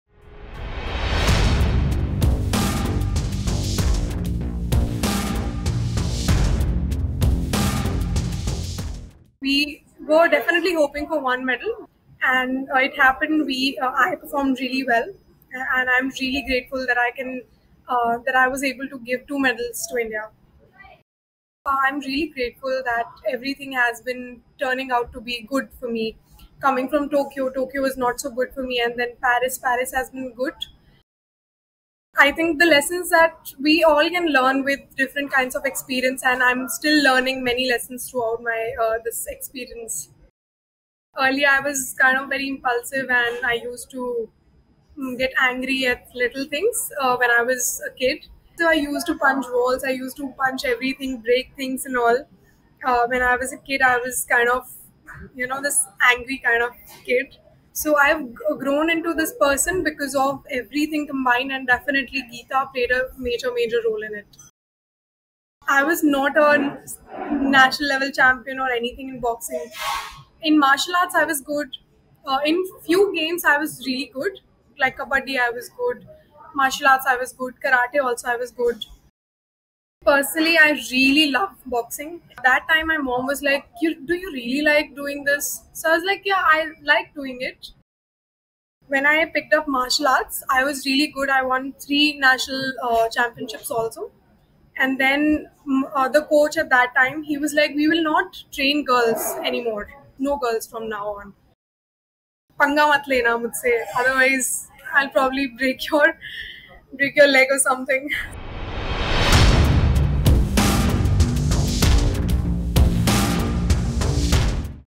'I Got Angry, Punched Walls, Broke Things': Manu Bhaker On Her Journey To Olympic Medals
In an exclusive interview with Hindustan Times' Kumkum Chadha, Olympic medalist Manu Bhaker opens up about the intense emotions and challenges she faced. She revealed moments of frustration when she reacted by punching walls and breaking things, shedding light on the struggles behind her path to success. Watch this excerpt from the full interview that airs at 11 AM on August 25 on Hindustan Times.

THE INTERVIEW